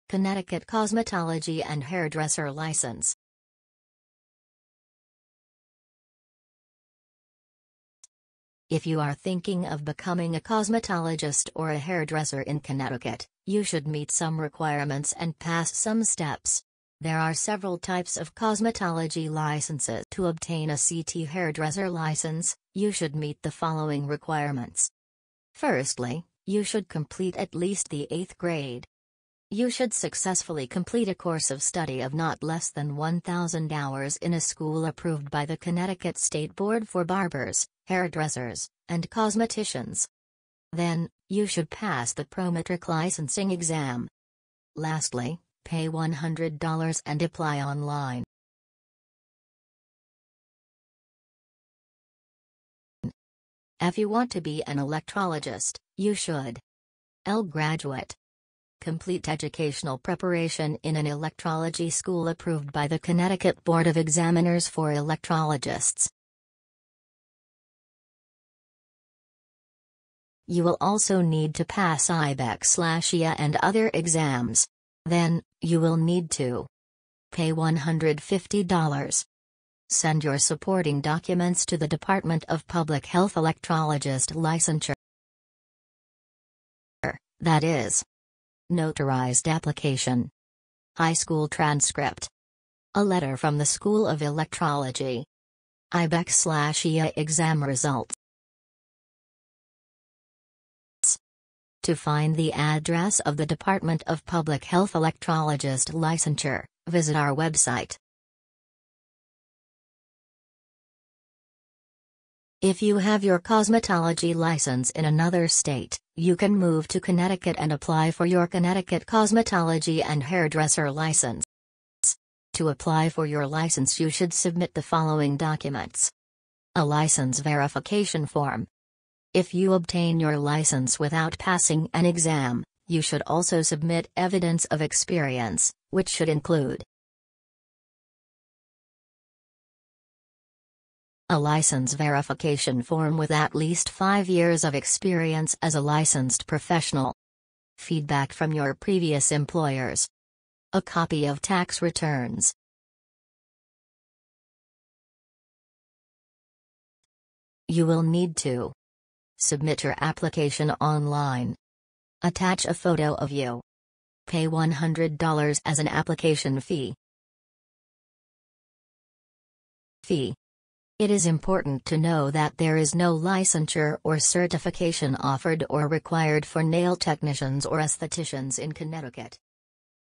Connecticut Cosmetology and Hairdresser License - What You need to get started
If you want to get a Connecticut Cosmetology and Hairdresser License, you should know there is no general license. Here you can find how to apply for your license.